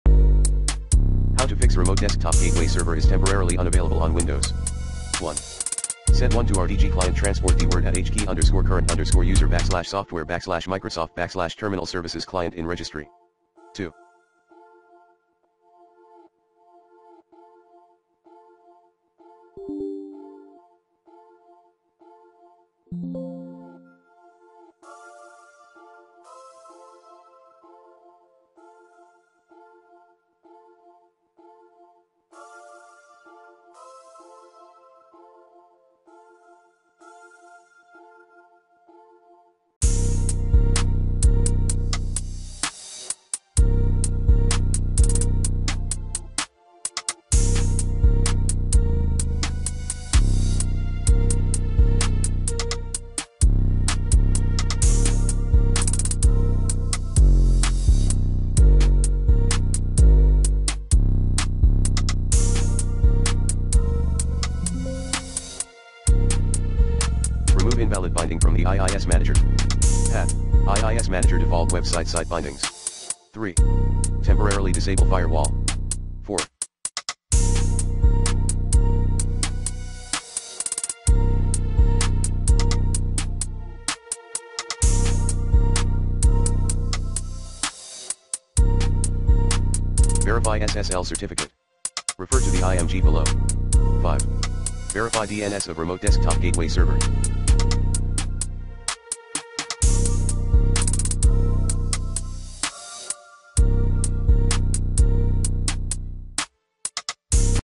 Fix Remote Desktop Gateway server is temporarily unavailable on Windows
Here's how to Fix Remote Desktop Gateway server is temporarily unavailable on Windows.